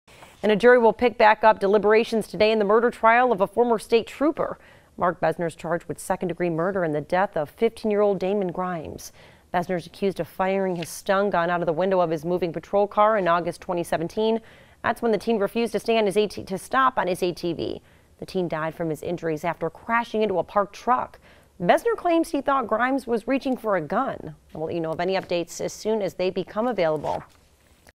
Jury set to pick back up deliberations in Bessner trial
A jury is scheduled to pick back up deliberations Wednesday in the murder trial for a former state trooper. Mark Bessner is charged with second-degree murder in the death of 15-year old Damon Grimes.